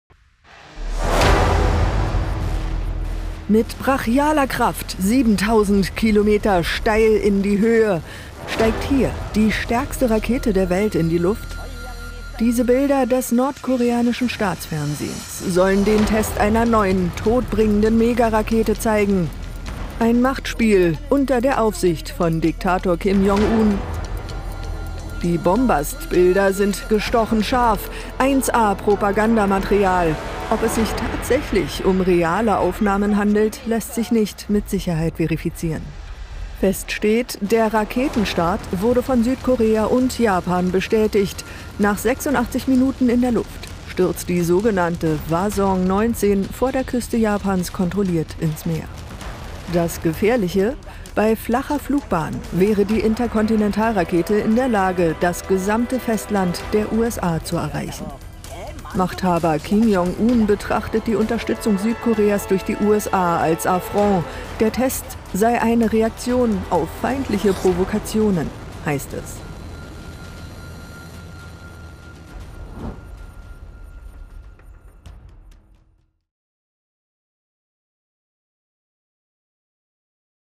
Nordkorea - Todbringende Monsterrakete unter Kim Jong Un's Aufsicht getestet.
Juhu, unser kleiner Dicker hat endlich wieder ein neues Spielzeug! 😡
Und sein Volk hungert!
(Quelle KRT Handout via Reuters / BILD).